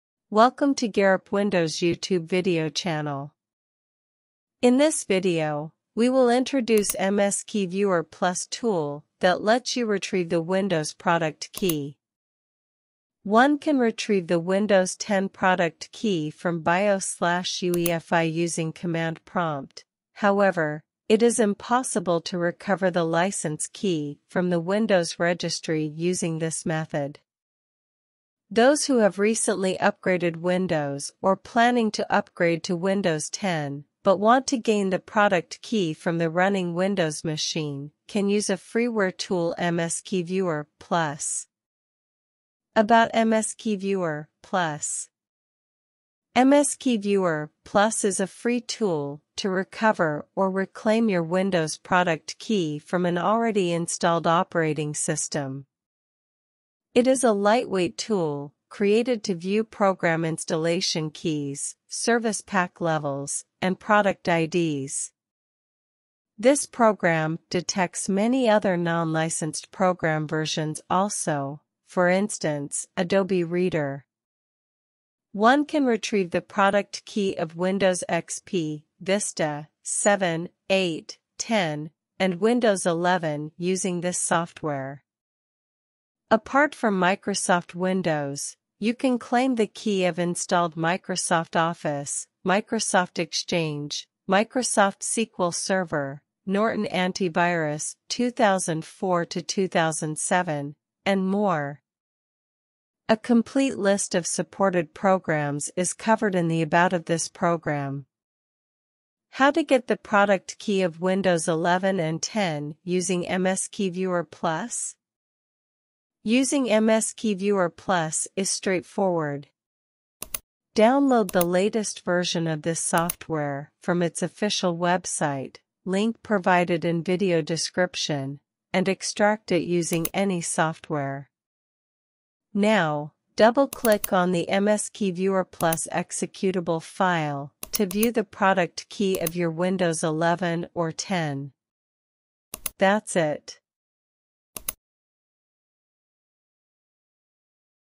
MSKeyViewer Plus Let’s Recover Windows 11 or 10 Product Key
In this video, we will introduce MSKeyViewer Plus tool that lets you retrieve the Windows product key.

One can retrieve the Windows 10 product key from BIOS/UEFI using Command Prompt; however, it is impossible to recover the license key from the Windows Registry using this method. Those who have recently upgraded Windows or planning to upgrade to Windows 10 but want to gain the product key from the running Windows machine can use a freeware tool MSKeyViewer Plus.

About MSKeyViewer Plus

MSKeyViewer Plus is a free tool to recover or reclaim your Windows product key from an already installed operating system. It is a lightweight tool created to view program installation keys, service pack levels, and product IDs. This program detects many other non-licensed program versions also, for instance, Adobe Reader. One can retrieve the product key of Windows XP, Vista, 7, 8, 10, and Windows 11 using this software.

Apart from Microsoft Windows, you can claim the key of installed Microsoft Office, Microsoft Exchange, Microsoft SQL Server, Norton AntiVirus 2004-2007, and more! A complete list of supported programs is covered in the About of this program.

How to get the product key of Windows 11/10 using MSKeyViewer Plus?

Using MSKeyViewer Plus is straightforward. Download the latest version of this software from its official and extract it using any software.

Now, double-click on the MSKeyViewerPlus executable file to view the product key of your Windows 11 or 10.

That's it.